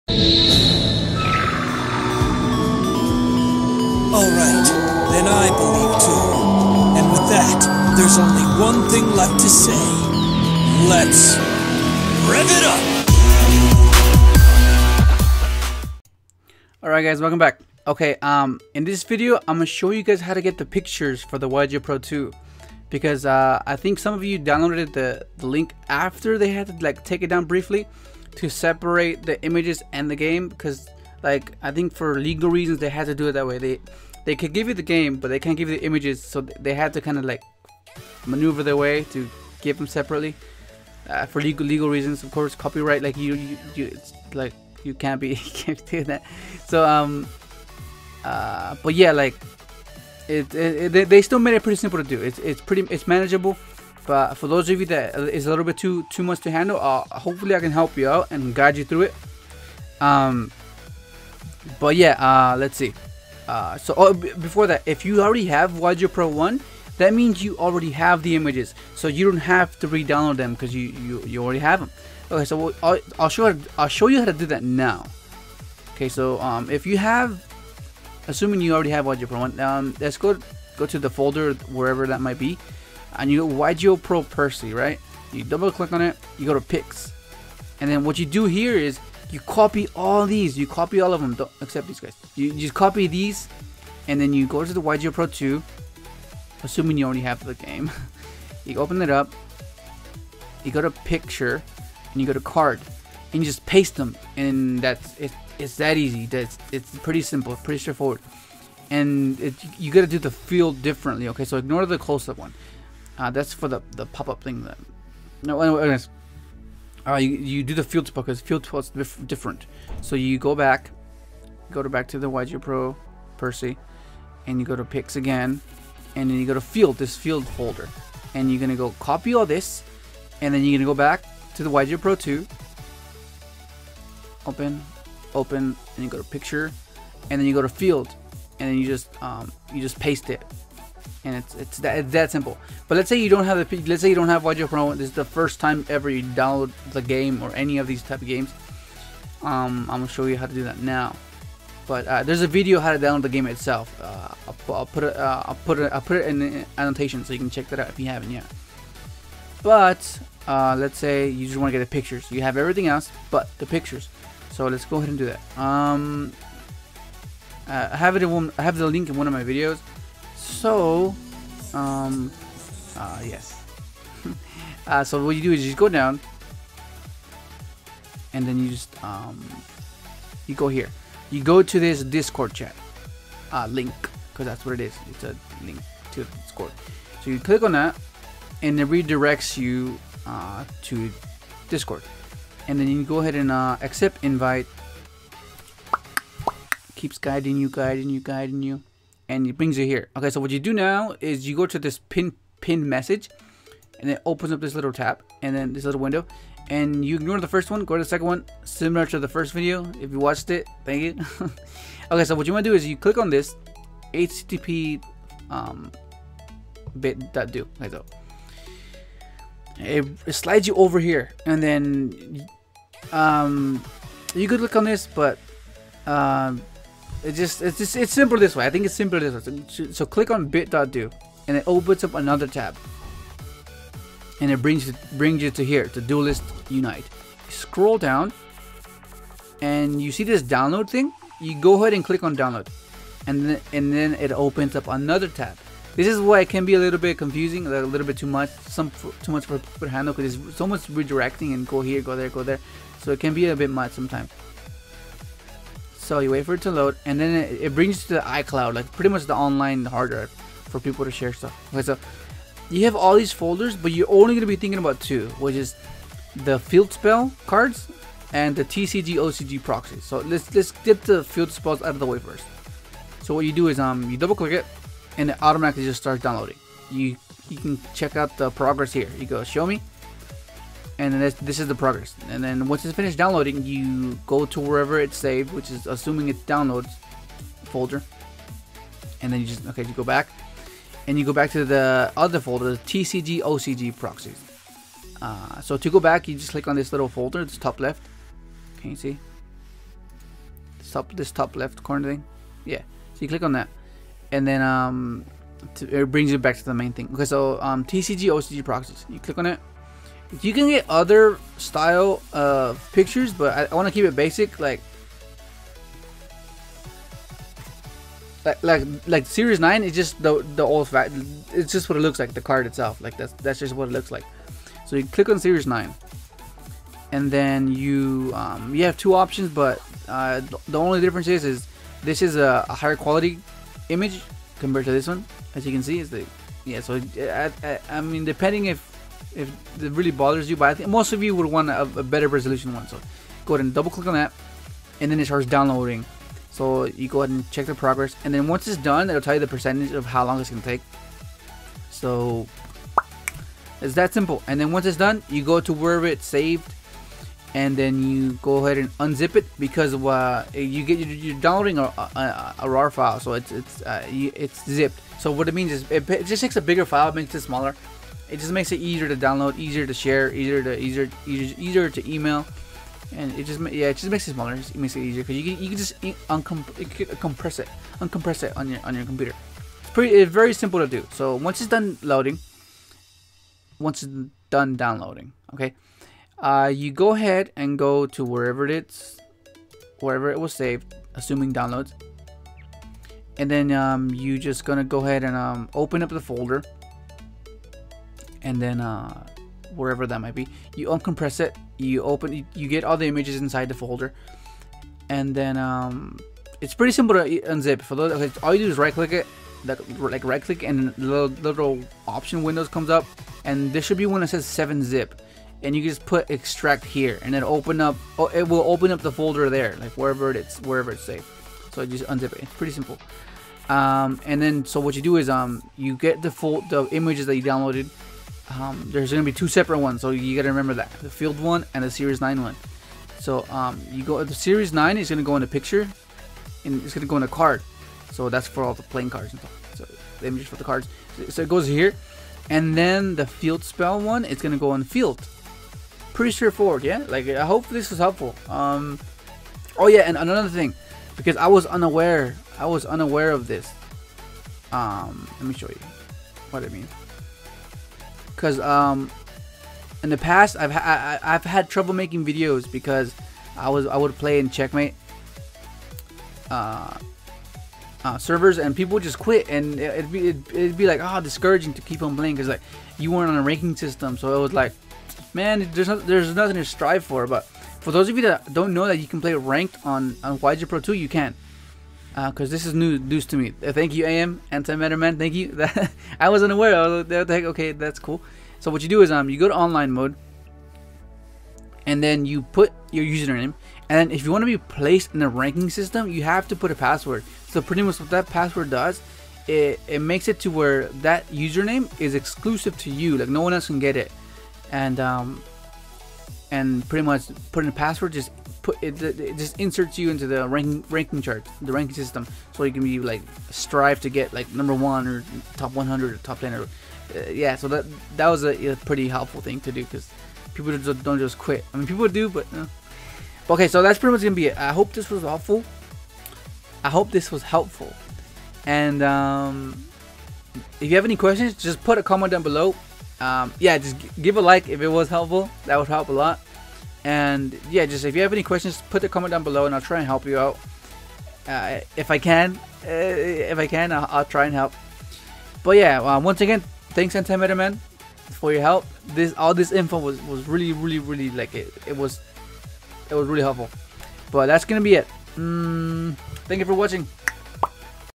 YGOPRO 2 No Pictures ( card images ) fix STEP by STEP
alright guys, some of you have ran into this issue.
i hope it can be of any help.

awesome hero
DARK SYNCHRO